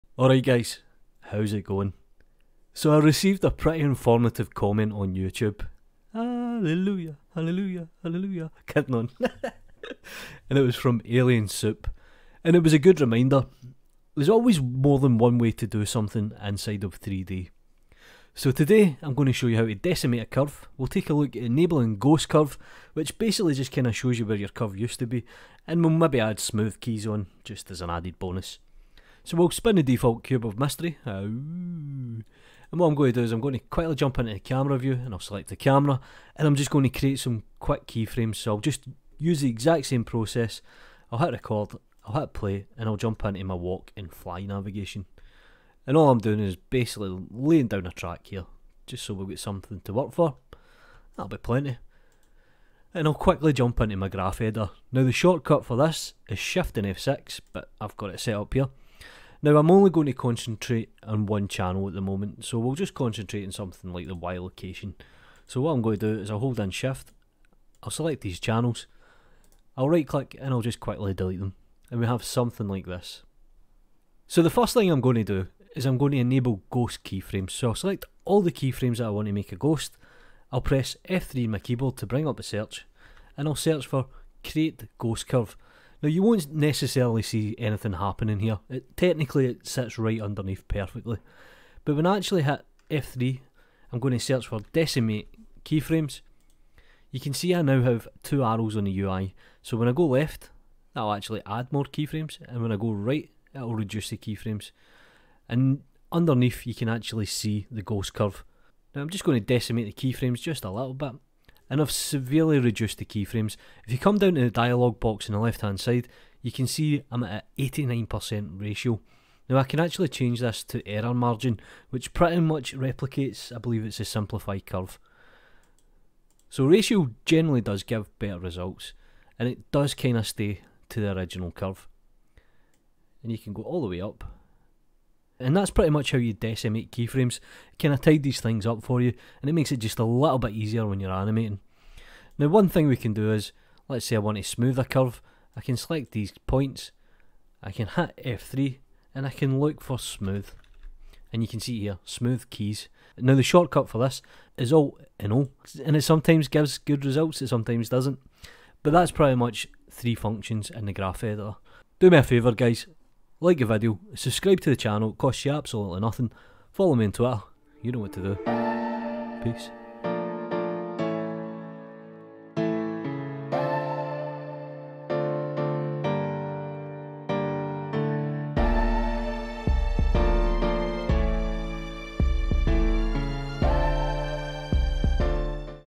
Blender 2.8 Tutorial: Decimate Keyframes & Create Ghost Curve
Revived a good comment on youtube in relationship to simplify curves video, today we look at how to decimate keyframes and create a ghost curve to keep track of original animation.